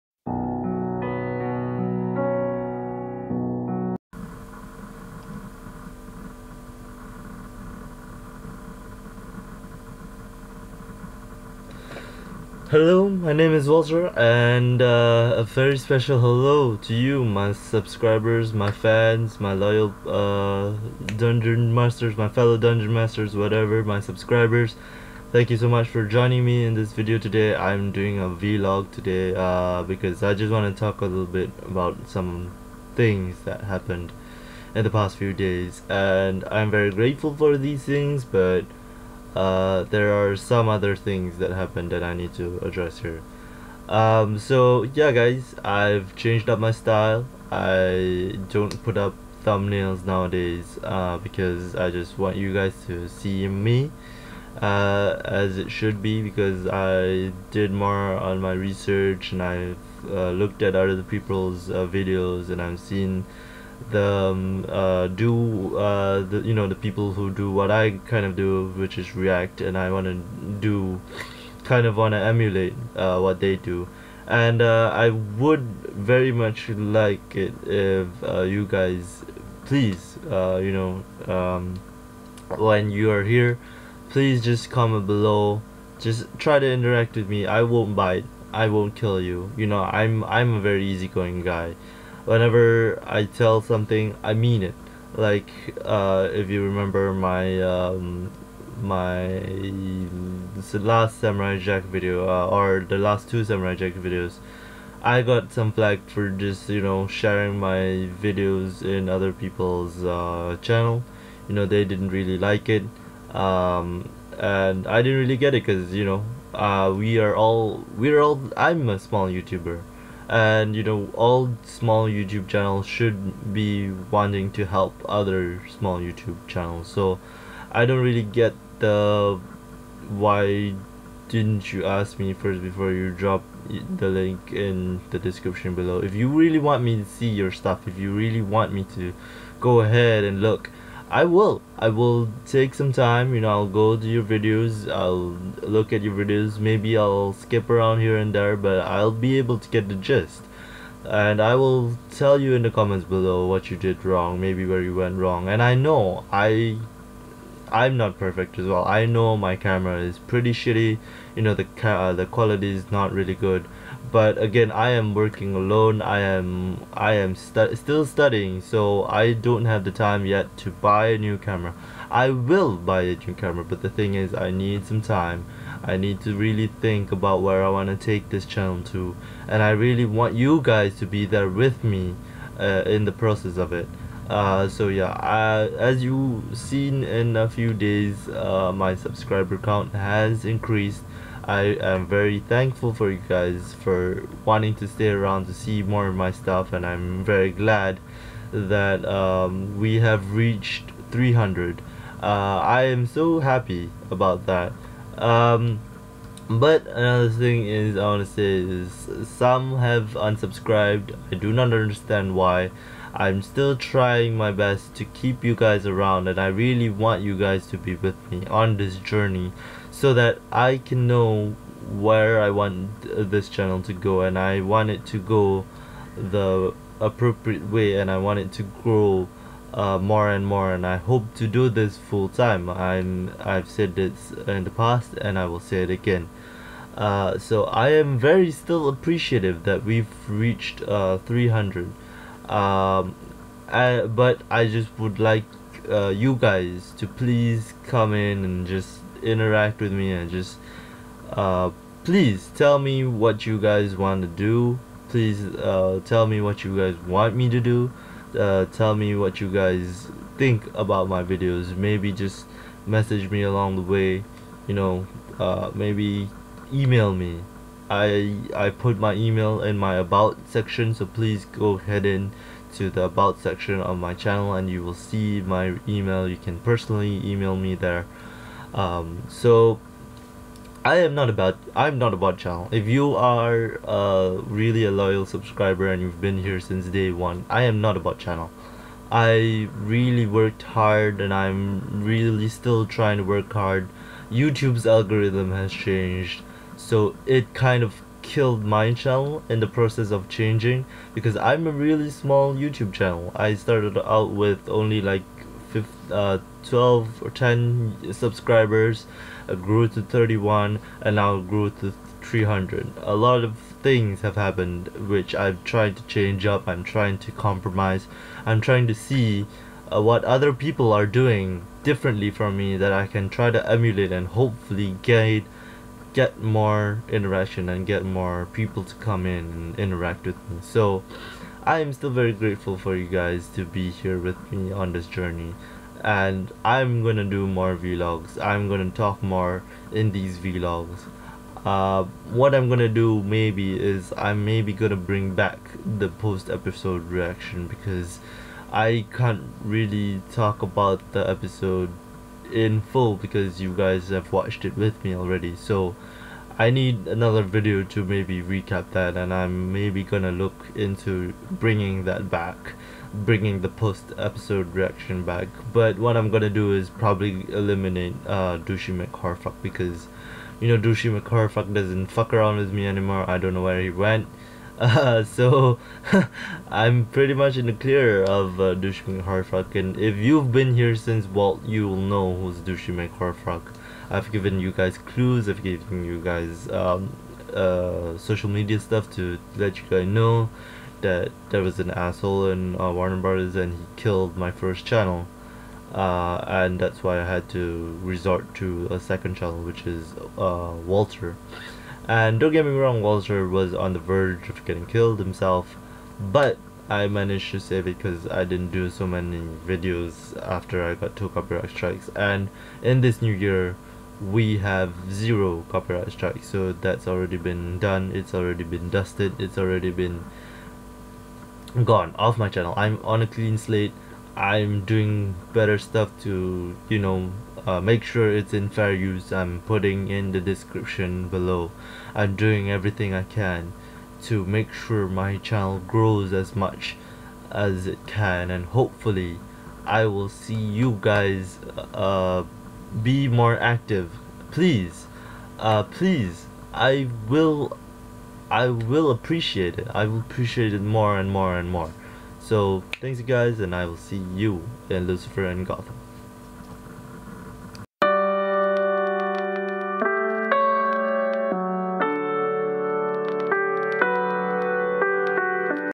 We Need To Talk WALTR's Vlogs| THANK YOU FOR 300 Subscribers!
WALTRVlogs So a lot of things have happened and I just wanna talk about them.
Don't worry I'm still doing reaction videos